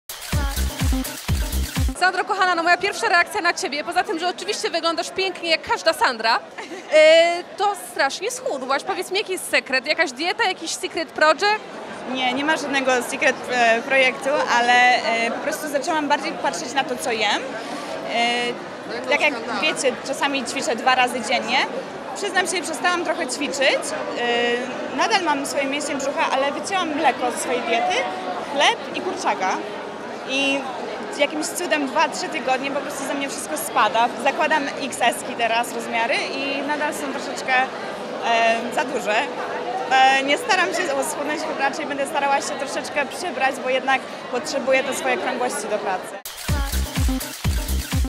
Kubicka o diecie: "Odstawiłam mleko, chleb i kurczaka. Noszę rozmiar XS, jest za duży"
Sandra Kubicka karierę modelki zaczęła w wieku 13 lat. Marzyła o pracy na wybiegu, by to osiągnąć musiała jednak schudnąć do wymaganej wagi. Skończyło się to omdleniem w wieku lat 17… podczas sesji zdjęciowej. W rozmowie z Pudelkiem przyznaje, że te czasy ma już za sobą. Obecnie nosi rozmiar XS, choć zdarza się, że jest za duży. Jaką dietę stosuje?